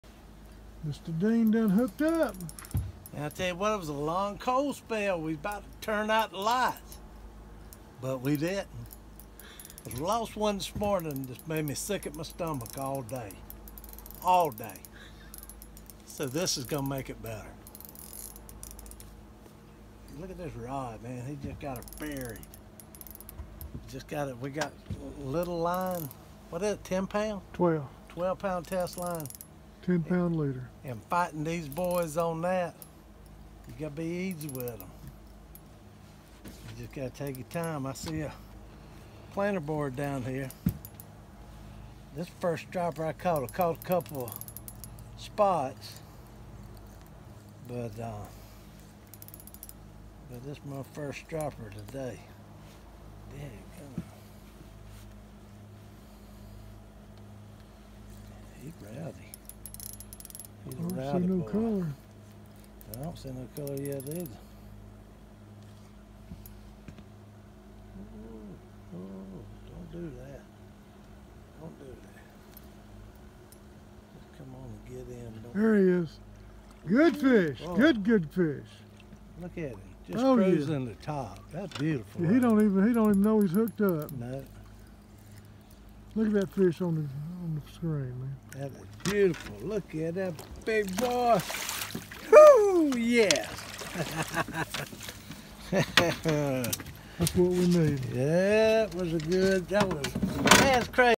Striper Fight!
Lake Hartwell battle between The Hat and this teenager.